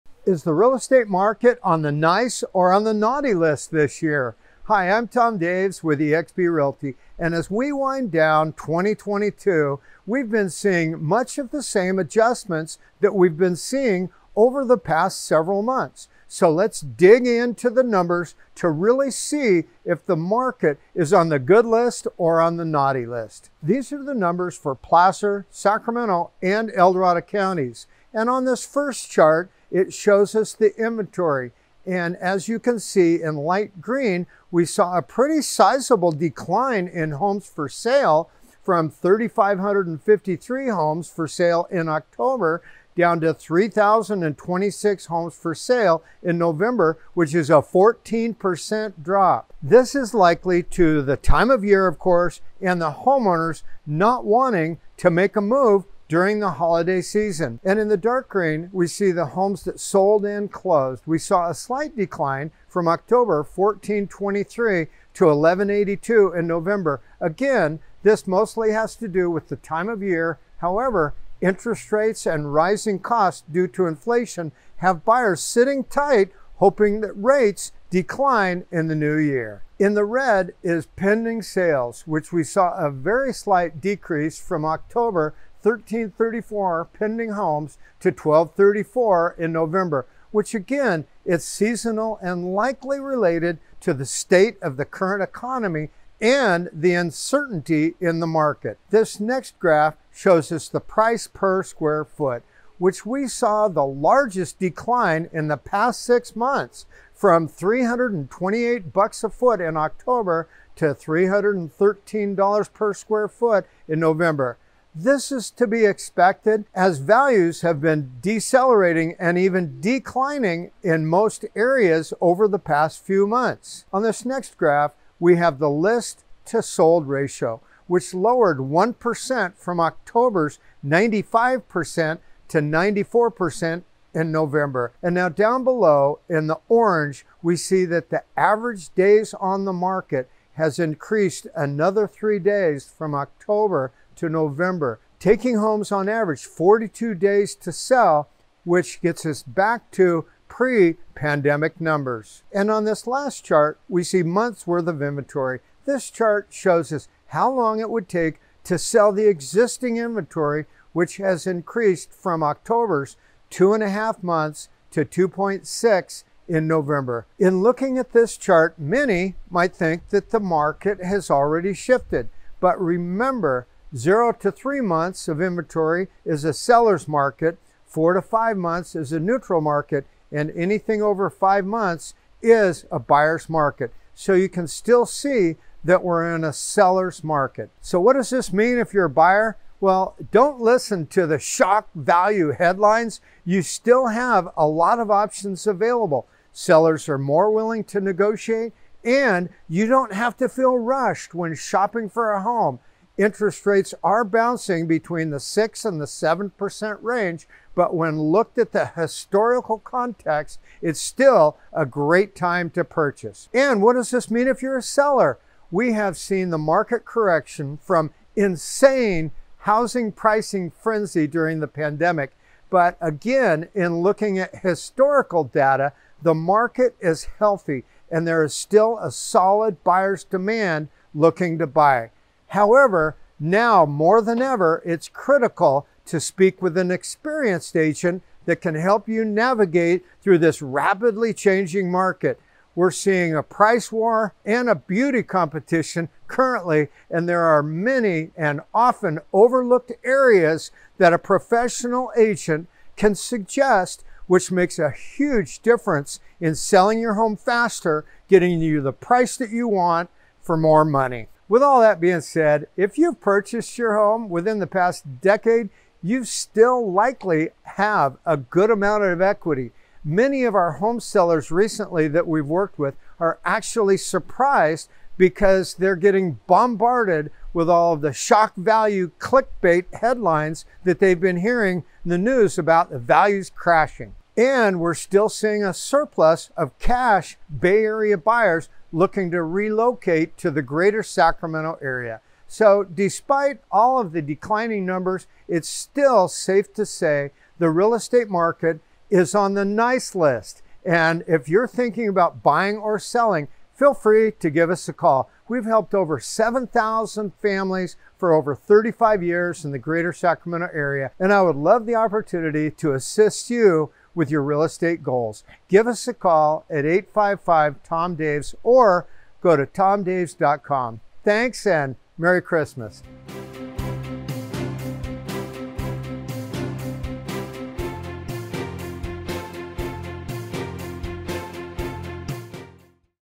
What’s Happening in Our Housing Market?
Christmas is right around the corner! This is a wonderful time of year to reflect on the past and where we want to go, so today I want to take a look at our housing market. You may have heard some pretty alarming headlines from the news, but is the state of our market really that bad? I’ll take a look at all the latest numbers and put them in context so that you can make the best real estate decision possible. To learn more about our market, check out this video.

Tom Daves
Tom Daves Team
Sacramento, California
CalBRE #00581837